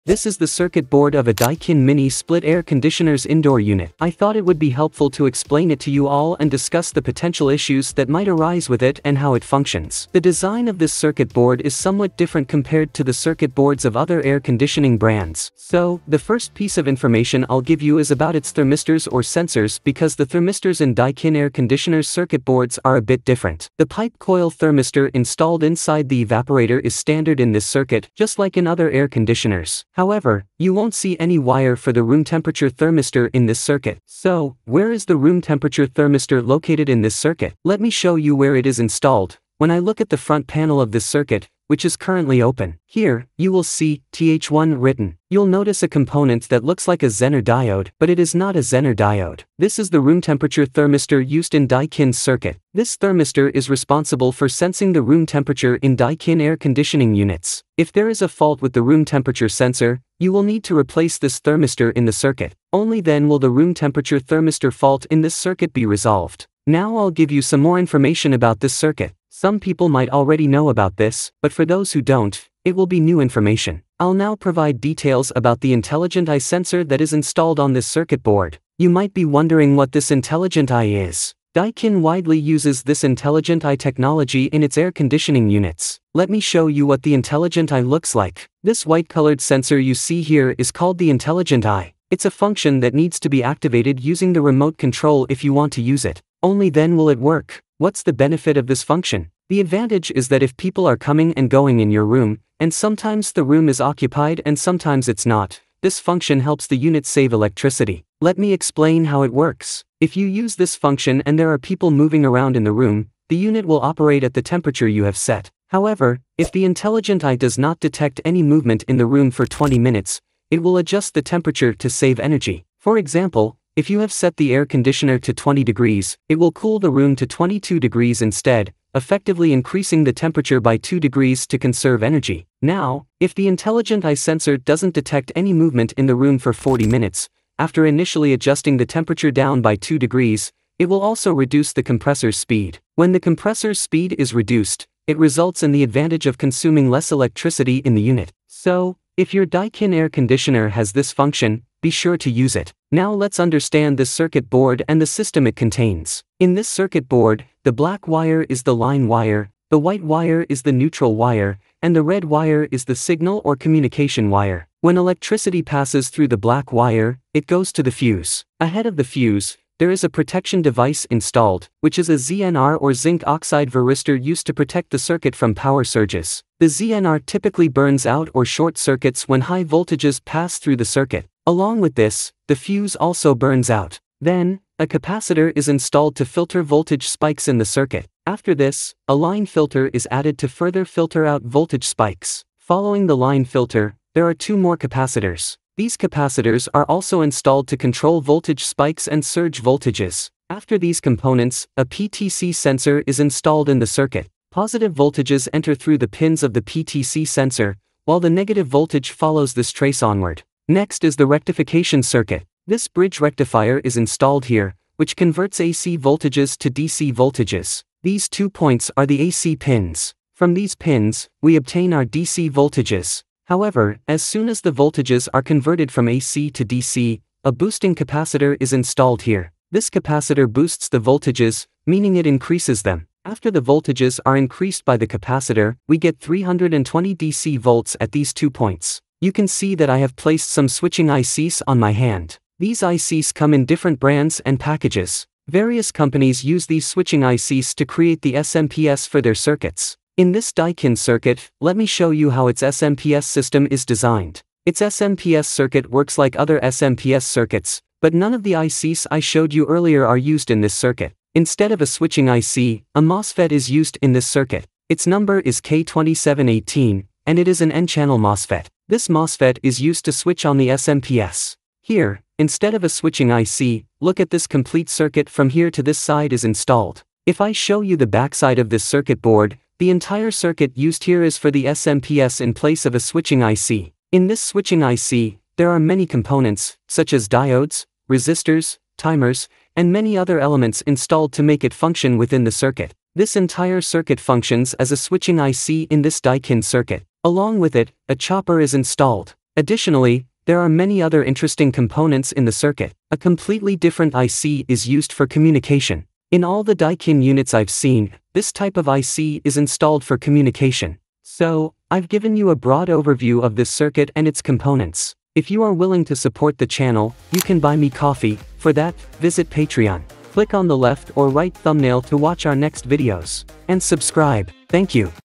Daikin Mini Split AC Indoor Unit Circuit Board Explained
Welcome to our in-depth guide on the Daikin Mini Split AC/Air Conditioner Indoor Unit Circuit Board! In this video, we'll explore the intricate details of the Daikin circuit board, including key components like thermistors, MOSFETs, and intelligent eye sensor technology. We'll cover common issues and troubleshooting tips, such as high voltage problems and component replacements. You'll also learn how the SMPS circuit and energy-saving features work to optimize your AC's performance. Whether you're an HVAC enthusiast, a technician, or a curious homeowner, this practical demonstration will help you understand and maintain your Daikin air conditioner.